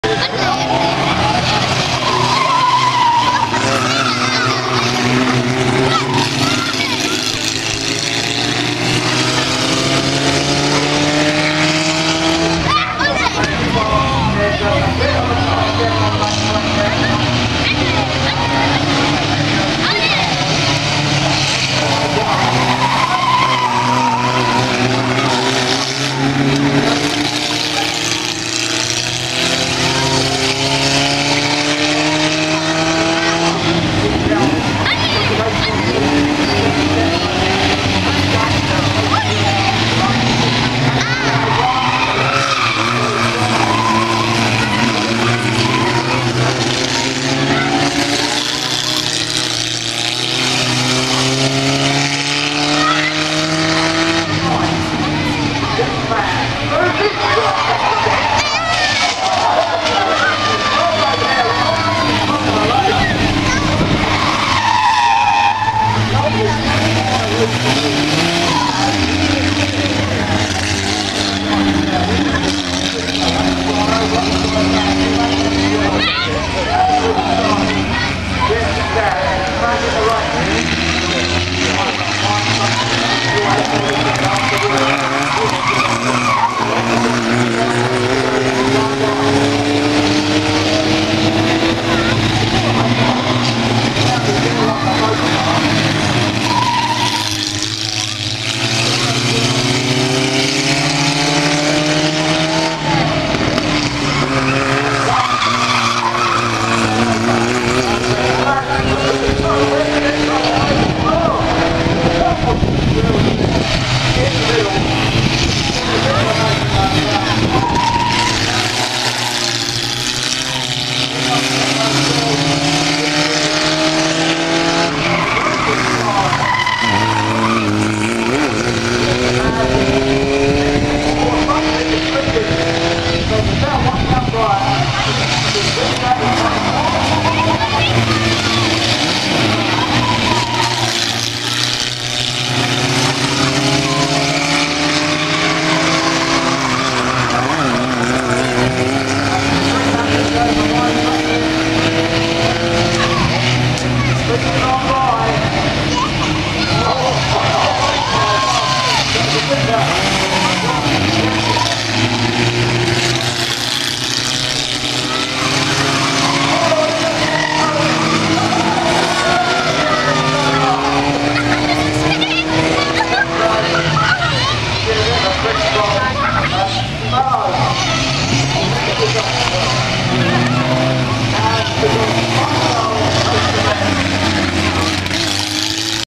Robin Reliant Banger Racing - Yarmouth Stadium | Made in Sheffield
Let's see if one flips!!

Robin Reliant Banger Racing - Yarmouth Stadium